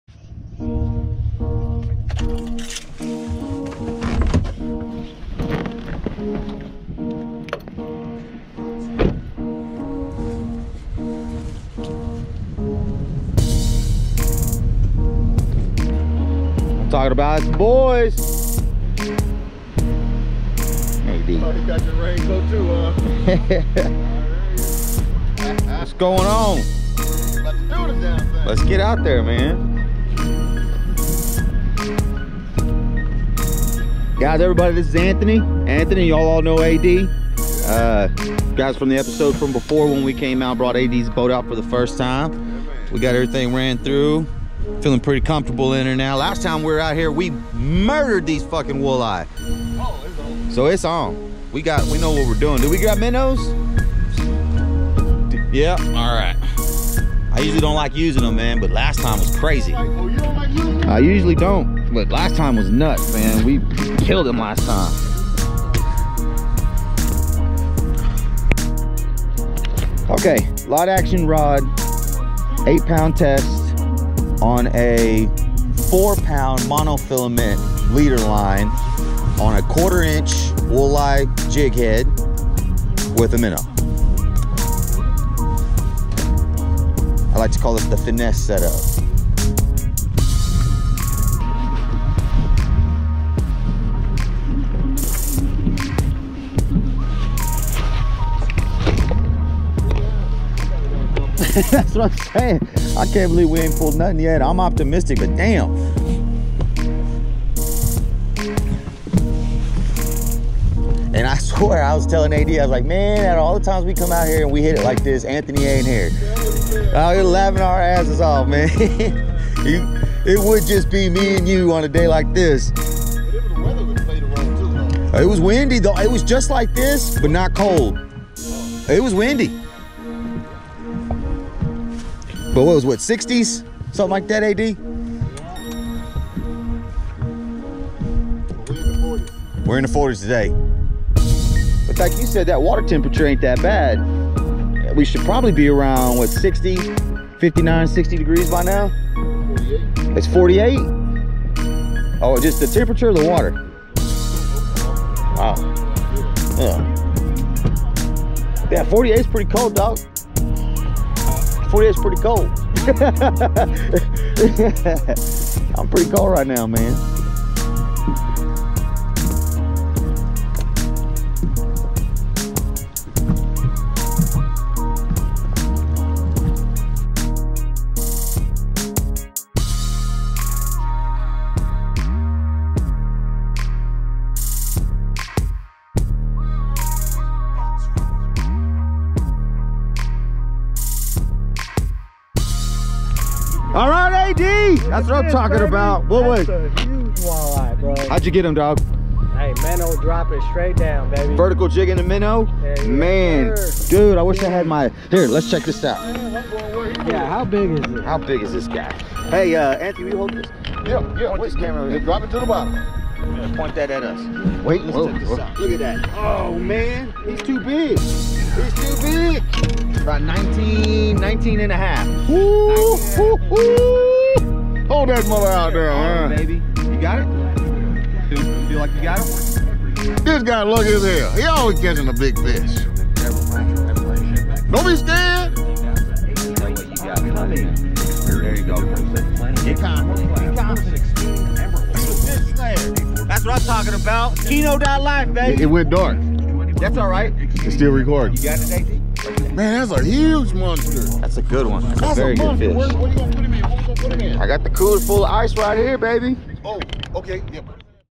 My Boys and I fishing at Lake Meredith
Anthony, AD and I fishing at Lake Meredith